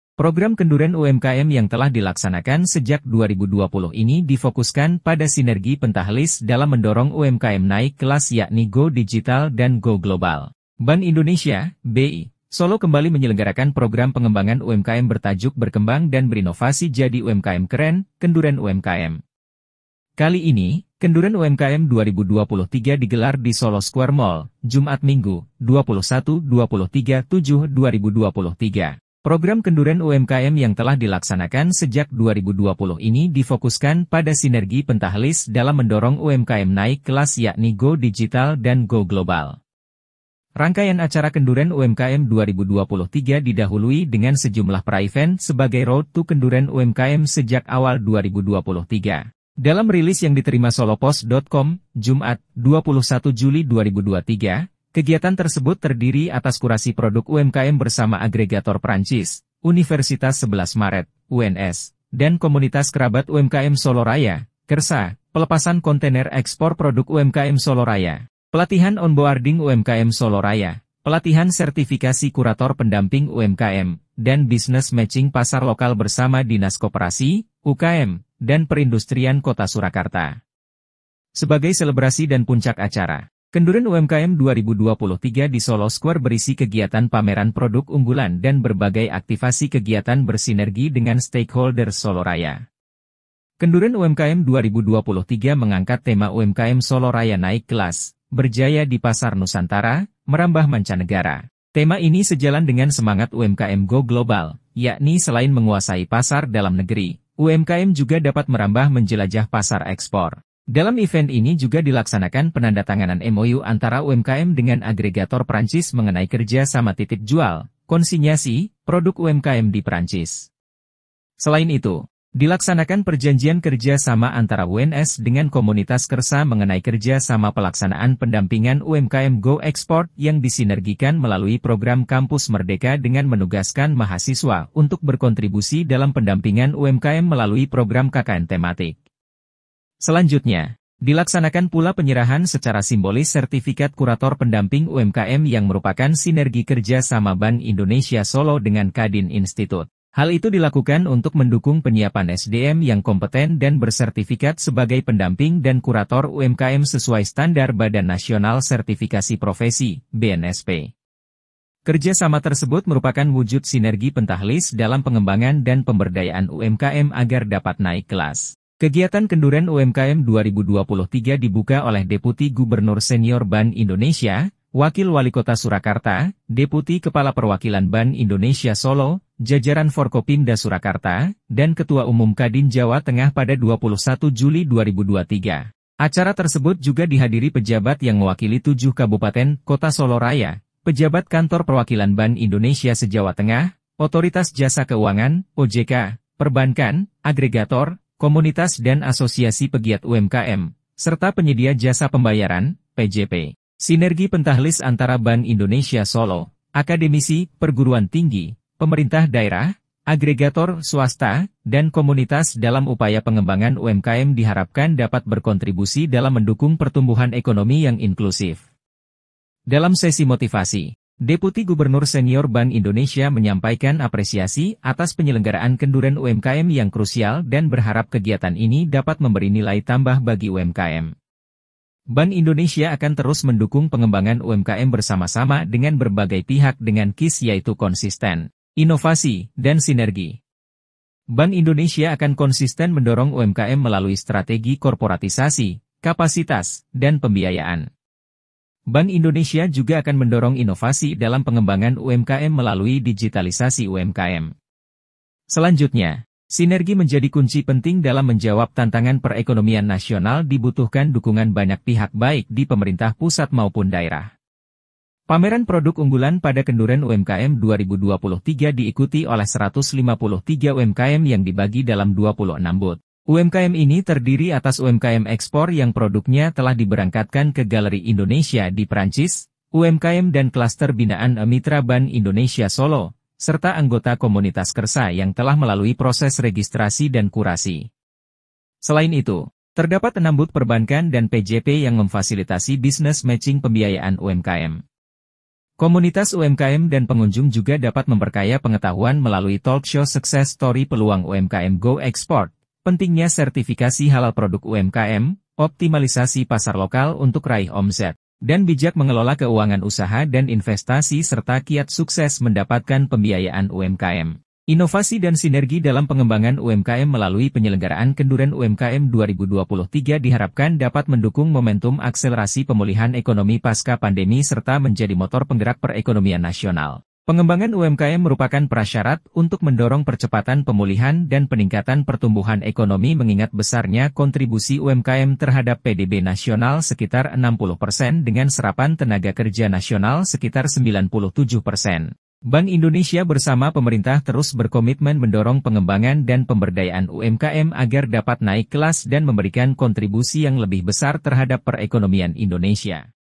Kenduren UMKM Oleh BI di Surakarta
005  Kenduren UMKM Oleh BI di Surakarta
Closing Ceremony KENDUREN UMKM SOLO RAYA 2023 | 23 JULI 2023
Puncak Hari UMKM Nasional Tahun 2023 digelar di Pamedan Mangkunegaran, Kota Solo, Jateng, Sabtu (12/8/2023).

Gubernur Jateng Ganjar mengungkapkan cara sukses membina 183181 unit UMKM yang berhasil mencatatkan Rp 38,9 triliun nilai transaksi, omset Rp 68,7 triliun, dan menyerap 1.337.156 tenaga kerja. Yakni dengan memfasilitasi mereka lewat pelatihan leveling.

Pelatihan levelling diadakan melalui Balai Latihan Koperasi UKM Jateng sebagai terobosan baru agar UMKM mampu mengikuti perkembangan usaha dan perekonomian sehingga lebih efektif dan dapat bersaing.